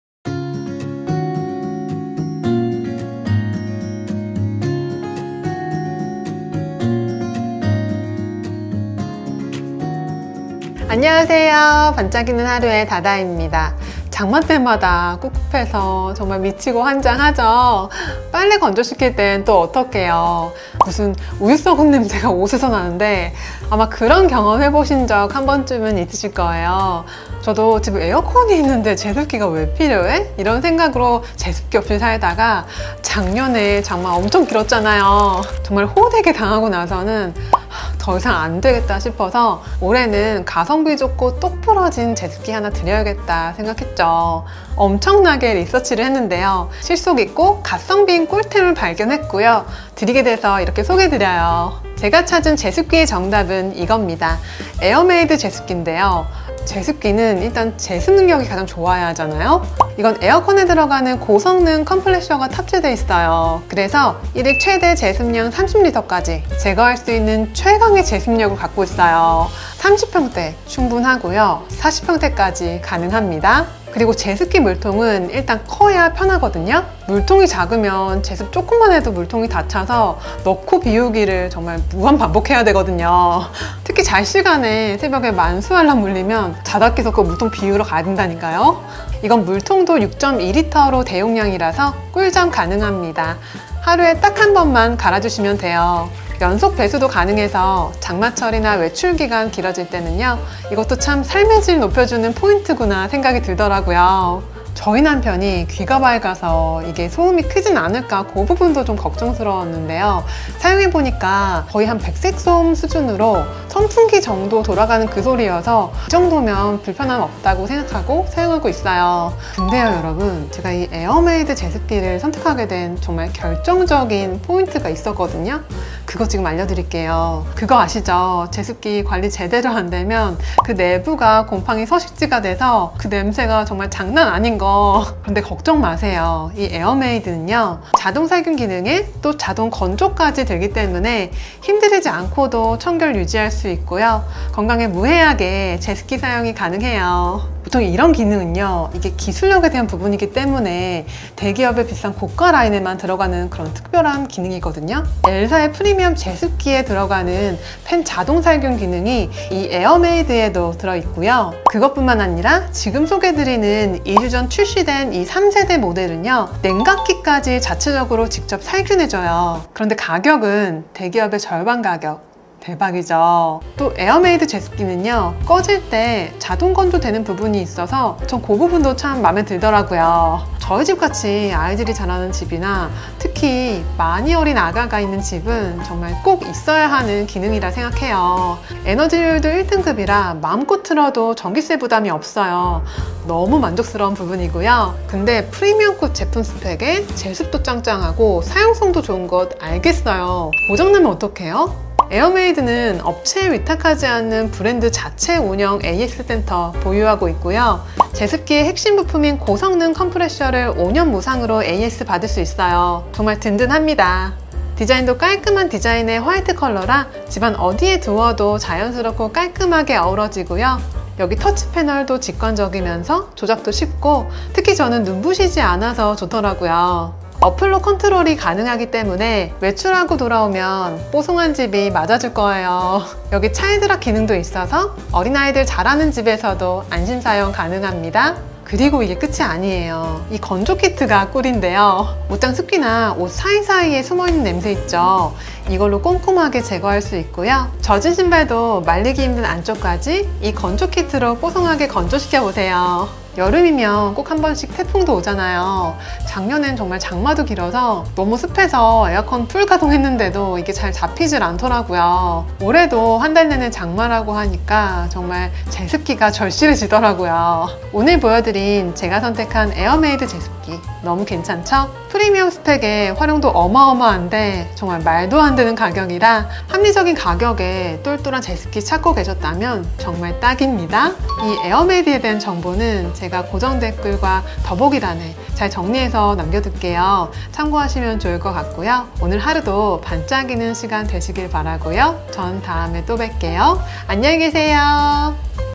[에어메이드제습기]더 늦기전에 들이세요.올여름 내내 삶의 질 책임집니다👆기관지 건강까지 챙긴 3세대 제습기. 팬 뿐만아니라 냉각기까지도 자동 살균 하세요
반짝이는 하루,dada 는
살림, 홈스타일링,라이프스타일에 대한 도움이 되는 정보를 함께 나누고 있어요.